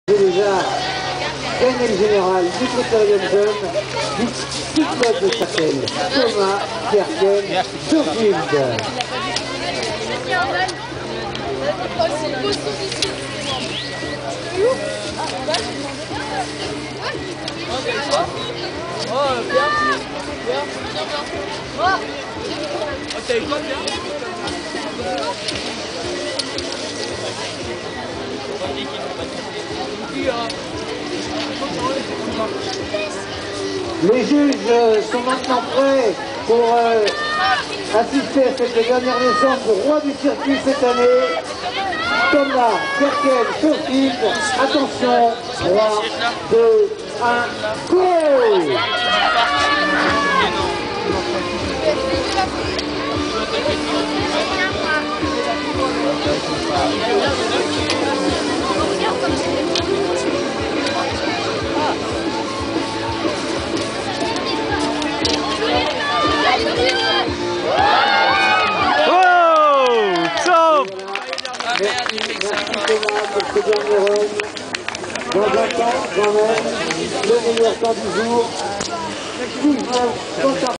Critérium Jeunes 2013 - 1st place
Thomas wins the last race of the season in Méribel, France. He also did his first Dspin in competition.